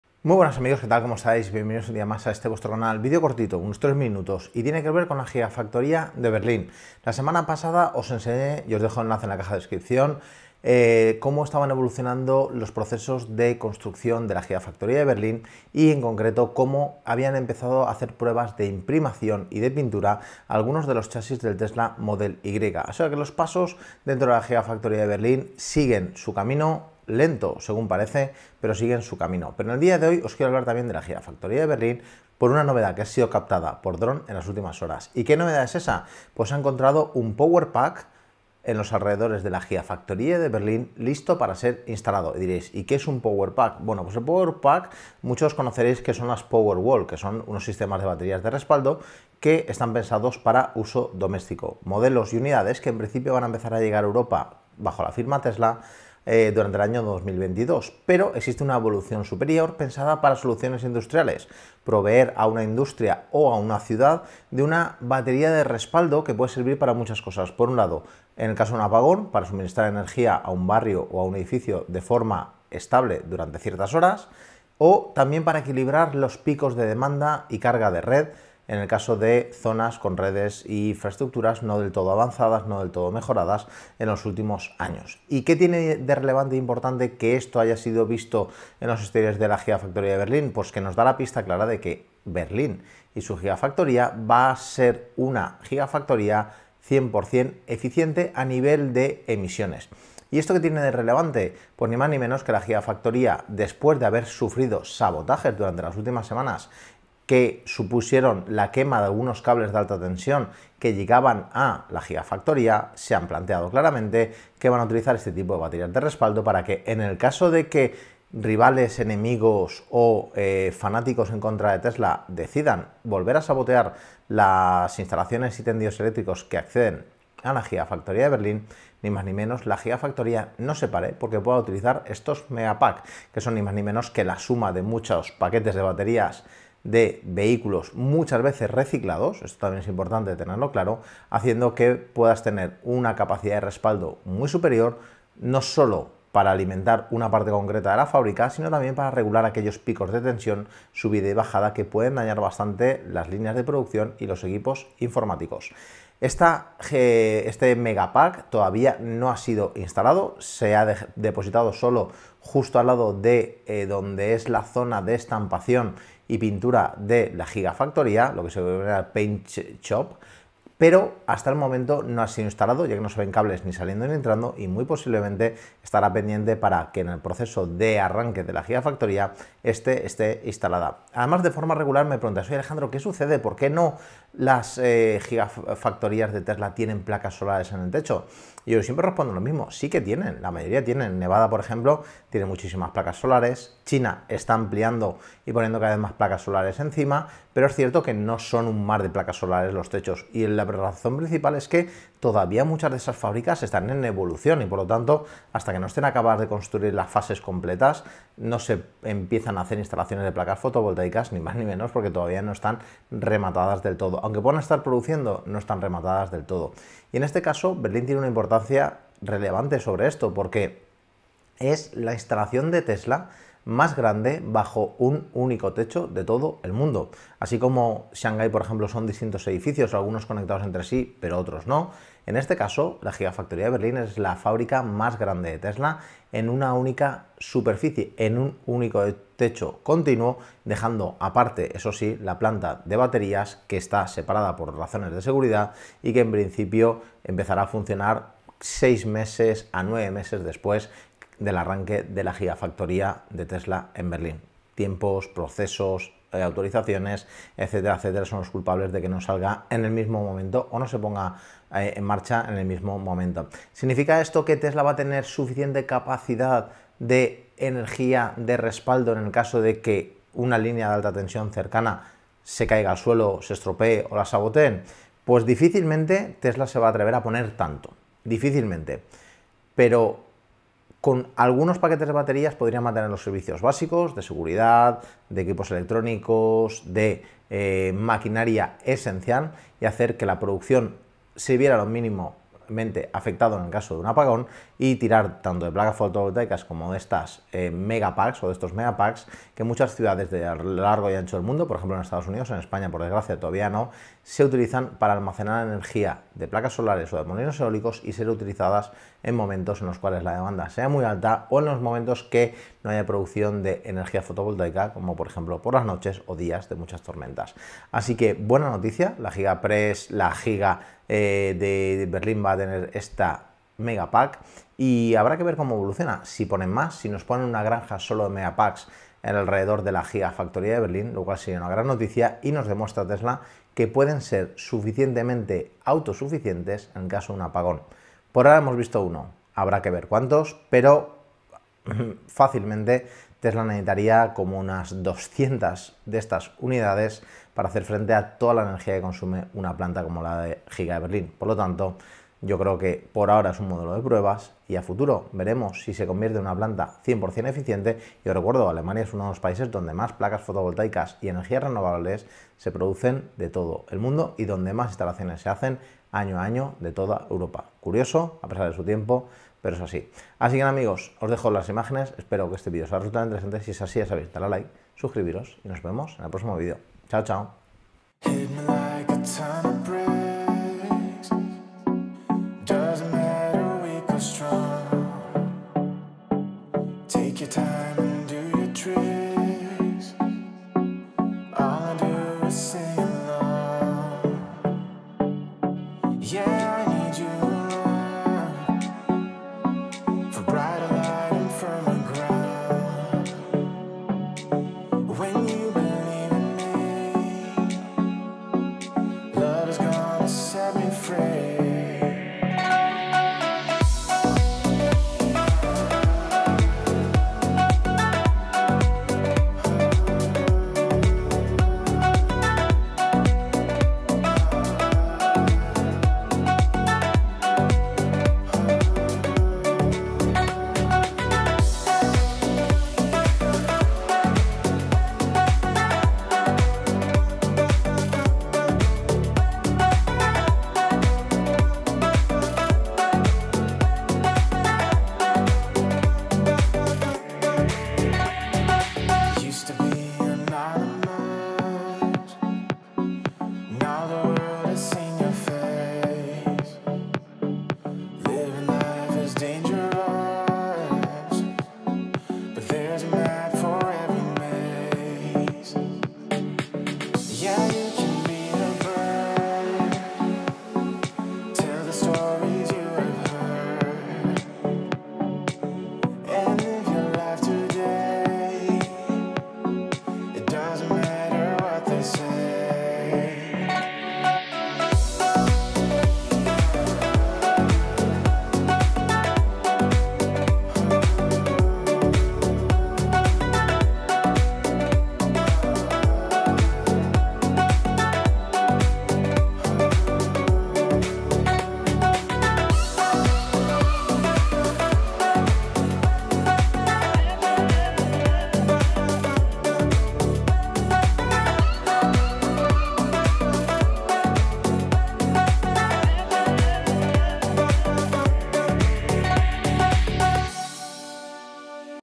Tesla Berlin Update: Power Pack listo para instalarse
La Giga Berlin sigue sus pasos para poder empezar a producir los primeros Tesla Model Y en el Q3 con suerte o para finales de 20021 pero los trabajos de instalación de equipo continuan y estos días han cazado vía Dron (enlace al final) como Tesla quiere hacer autosuficiente la Gigafactory.

Este Mega Pack podría ser el primero de mucho otros para hacer que Berlin pueda seguir trabajando aunque le corten la electricidad.